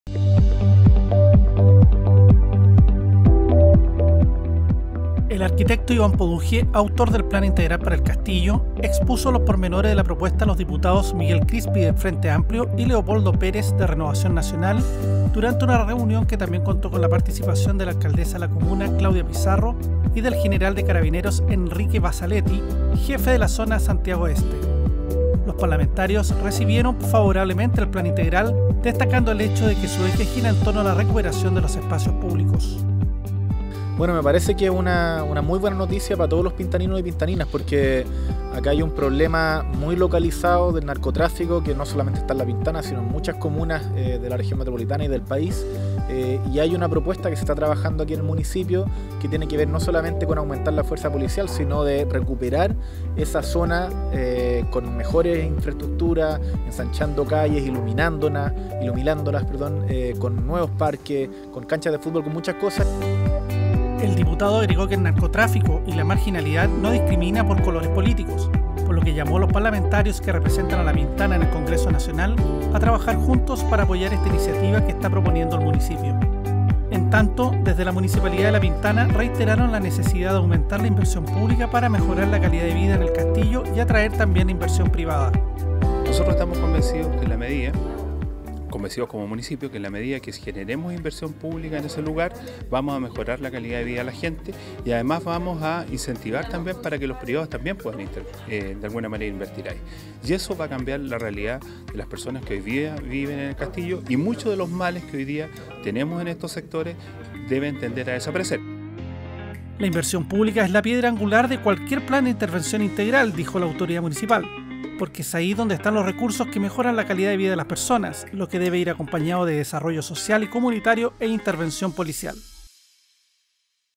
Diputados Crispi y Perez respaldaron Plan Integral para El Castillo
Con presencia de dos diputados del distrito fue presentado a los parlamentarios que representan a La Pintana el nuevo plan integral para el castillo